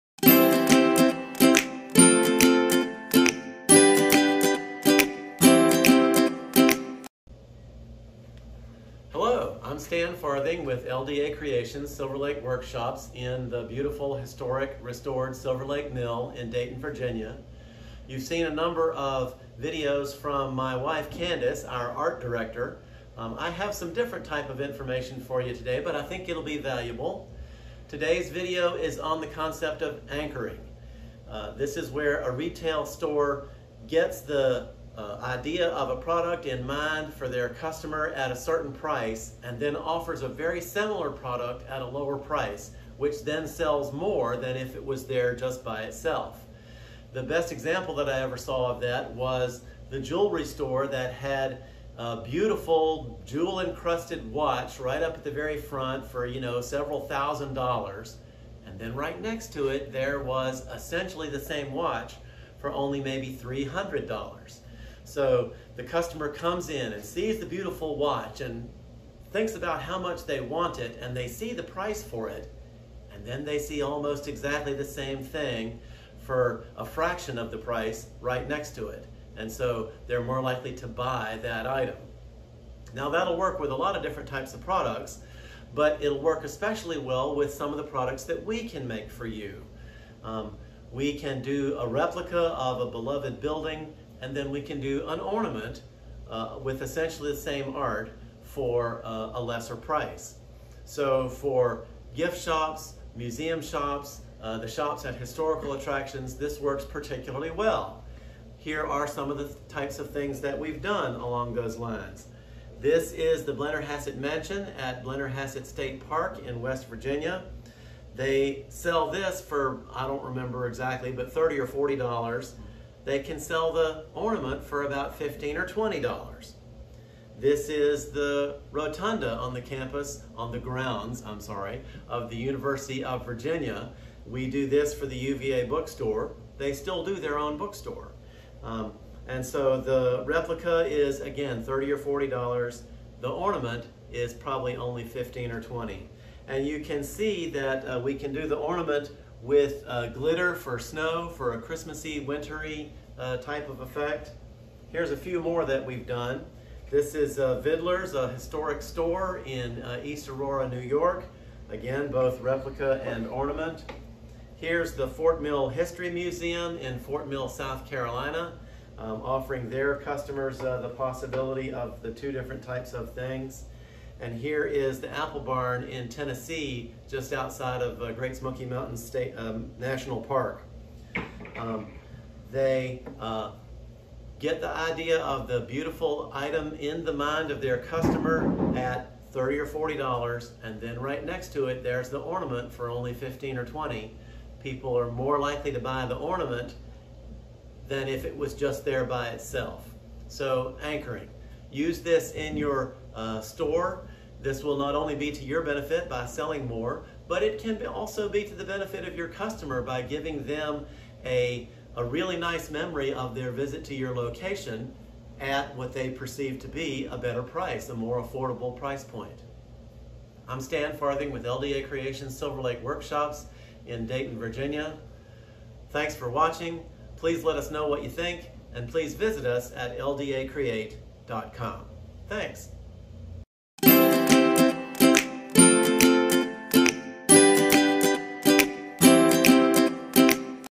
Anchoring
Gift shop retailers: have you wondered how to move more of your expensive but slower moving merchandise? This week, Stan Farthing, president of LDA creations will be sharing some tips on how to do just that!